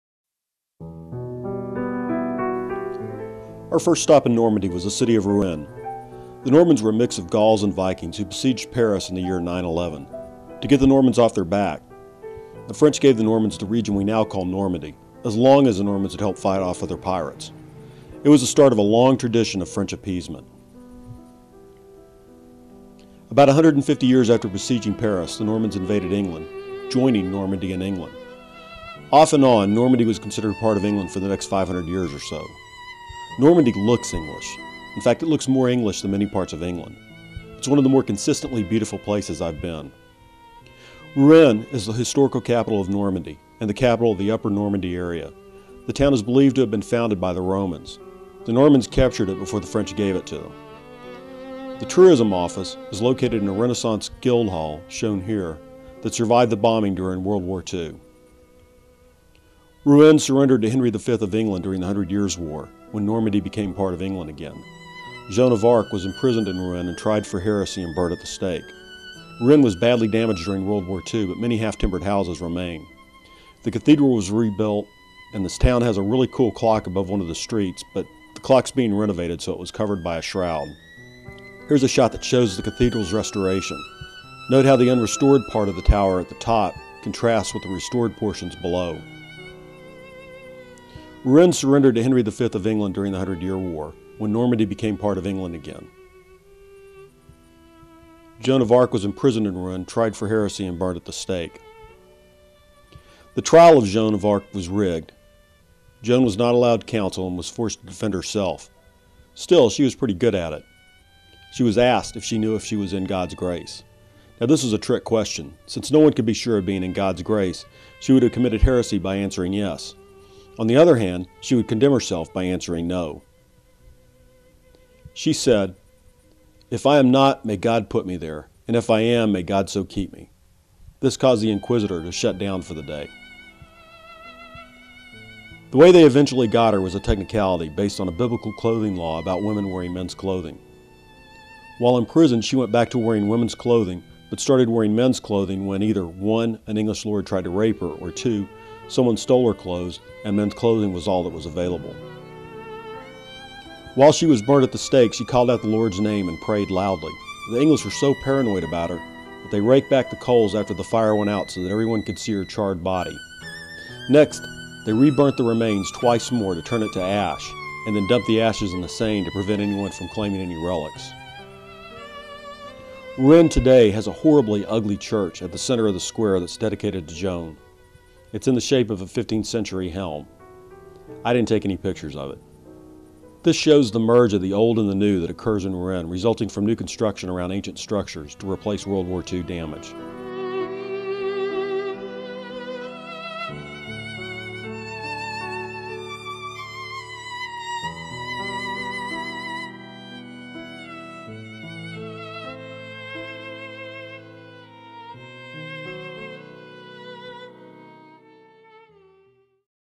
Rouen
This is one of a series of videos I prepared from still photographs during a trip to France. I've uploaded it for friends who are planning to visit France.